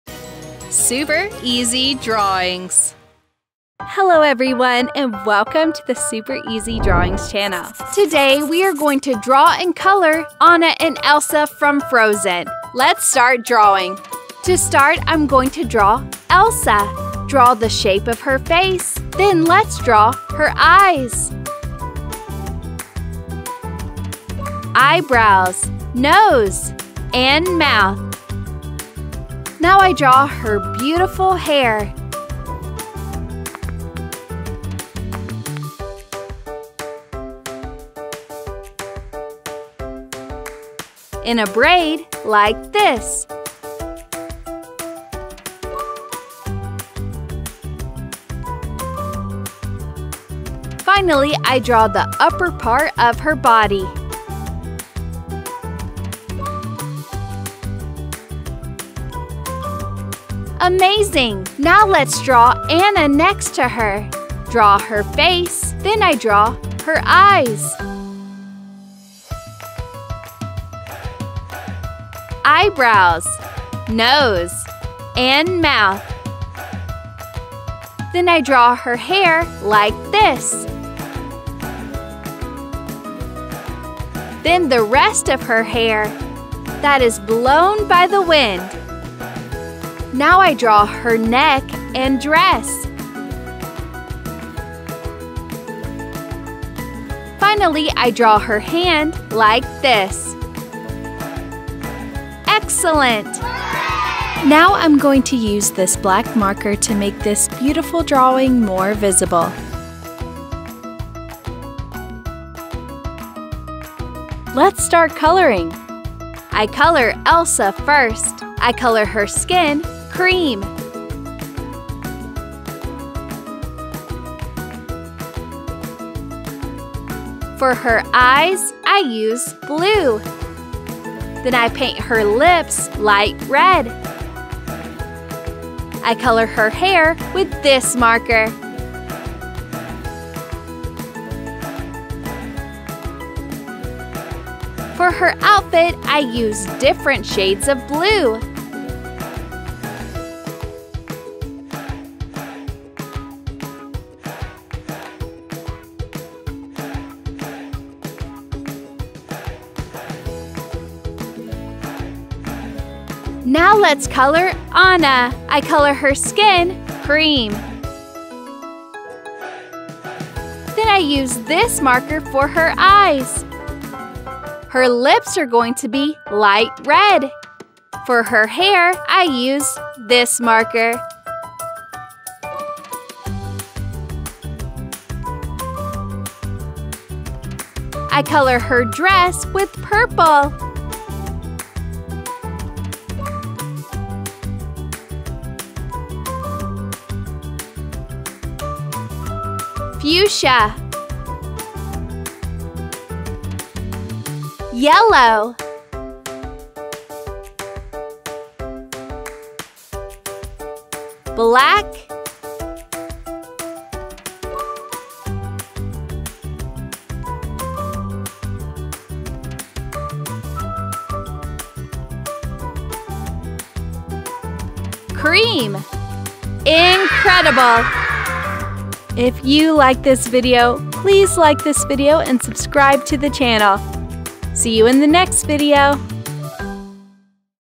Drawing and Coloring Elsa and Anna from Frozen ❄️👸🏼💙👸❄️ Drawings for Kids
Hi Everyone,
In This Video I Show You How To Draw and Color Elsa and Anna from Frozen ❄️👸🏼💙👸❄️.
Follow My Step By Step Drawing Tutorial And Make Your Own Elsa and Anna from Frozen Drawing Easy!